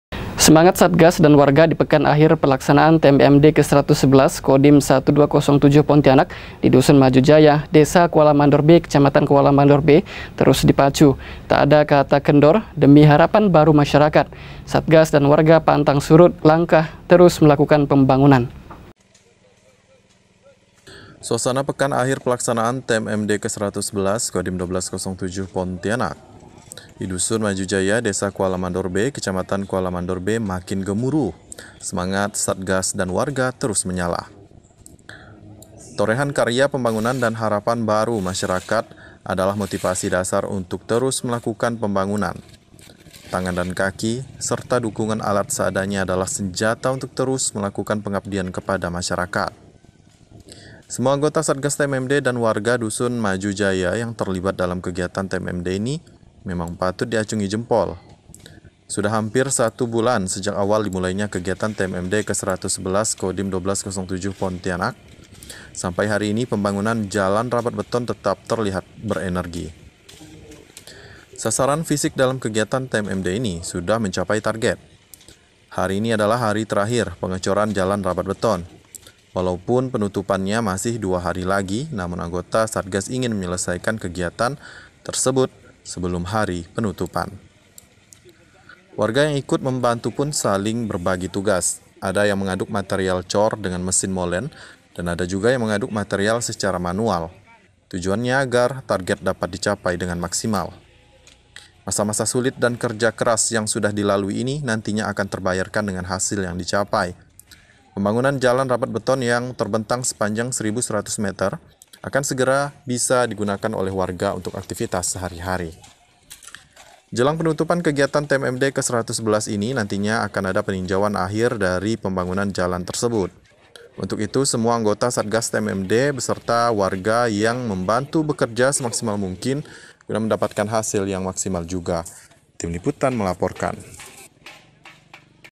HARI TERAKHIR, SEMANGAT SATGAS TMMD 111 KODIM 1207 DAN WARGA TERUS DIPACU
SEMANGAT SATGAS DAN WARGA DI PEKAN AKHIR PELAKSANAAN TMMD KE 111 KODIM 1207 PONTIANAK DI DUSUN MAJU JAYA, DESA KUALA MANDOR B, KECAMATAN KUALA MANDOR B TERUS DIPACU. TAK ADA KATA KENDOR, DEMI HARAPAN BARU MASYARAKAT, SATGAS DAN WARGA PANTANG SURUT LANGKAH TERUS MELAKUKAN PEMBANGUNAN.